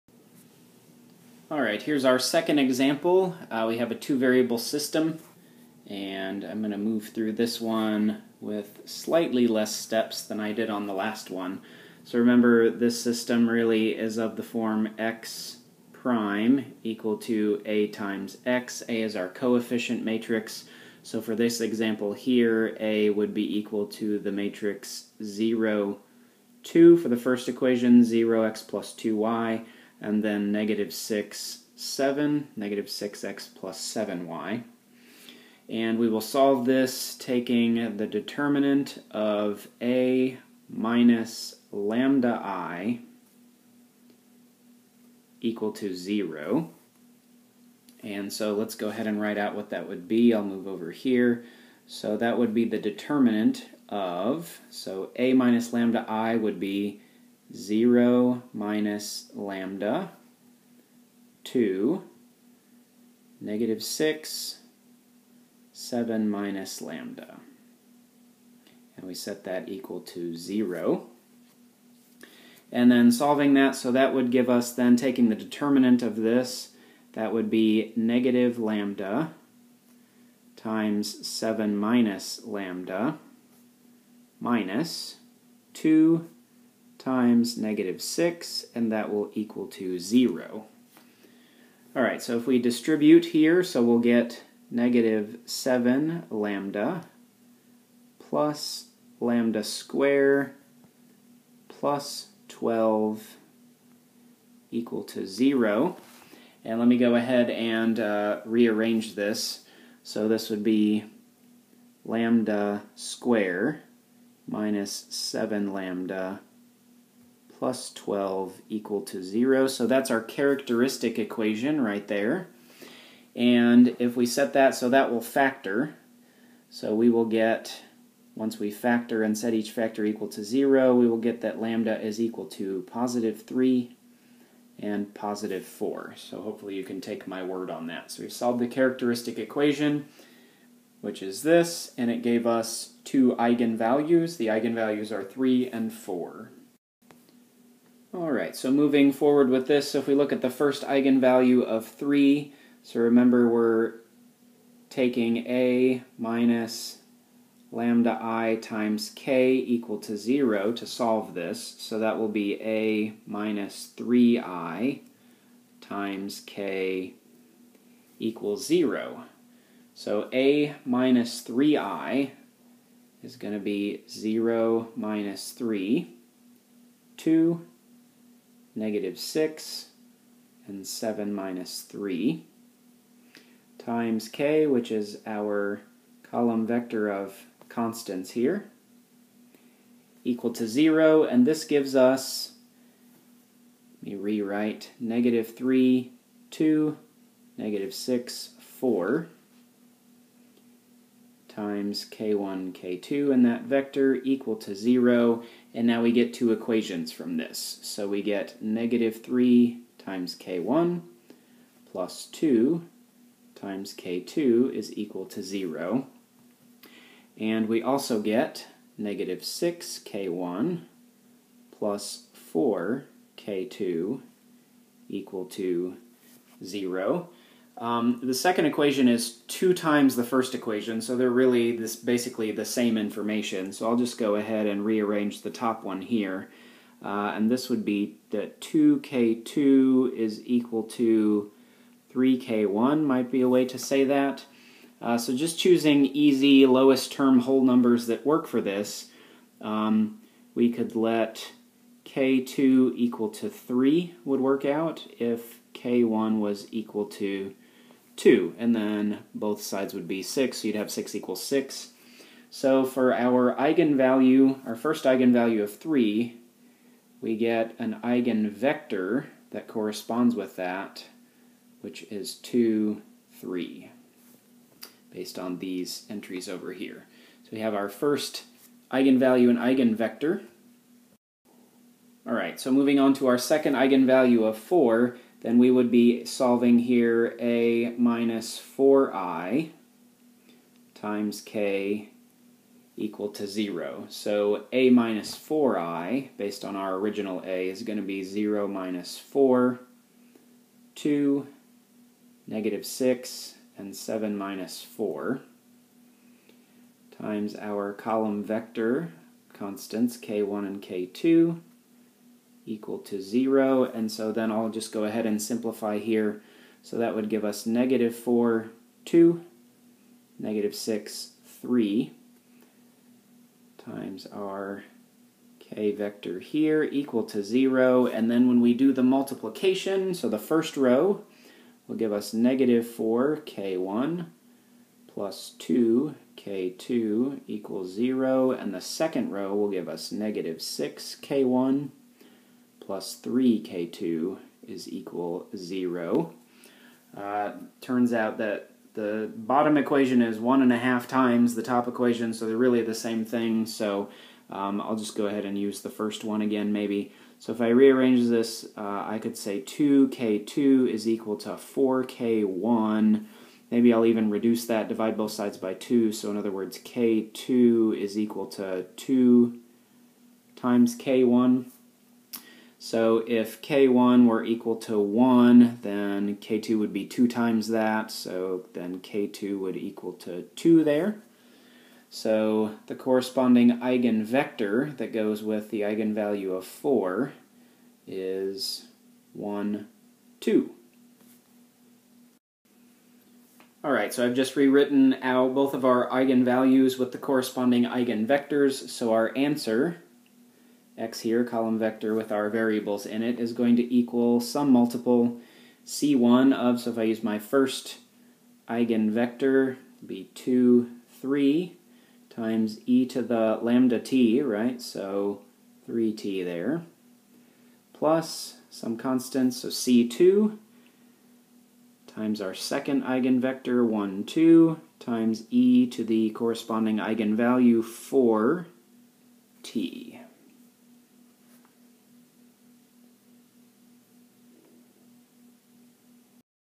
Solving Linear Systems with Eigenvalue/Eigenvector Method - Example 2
Shows another entire solution process of a 2-variable system using characteristic equation, eigenvalues, and eigenvectors.